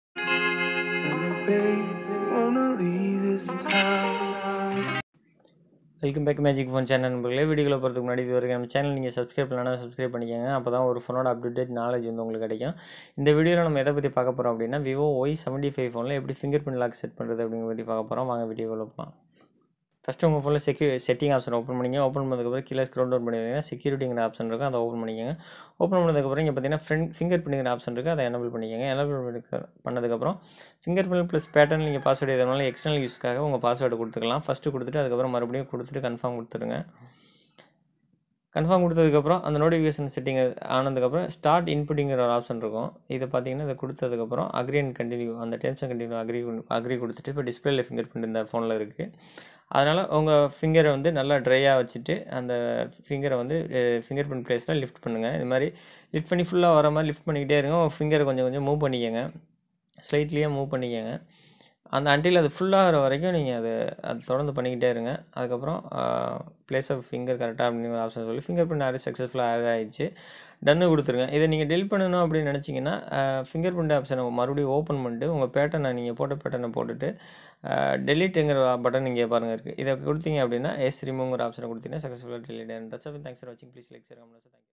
How to Set Finger Print Lock in VIVO Y75 in Tamil| How to set Vivo Y75 fingerprint lock and unlock
In today's video i am going to show How to Set Finger Print Lock in VIVO Y75 in Tamil phone full guide watch full video for acqurecy. unless you have to face a problem..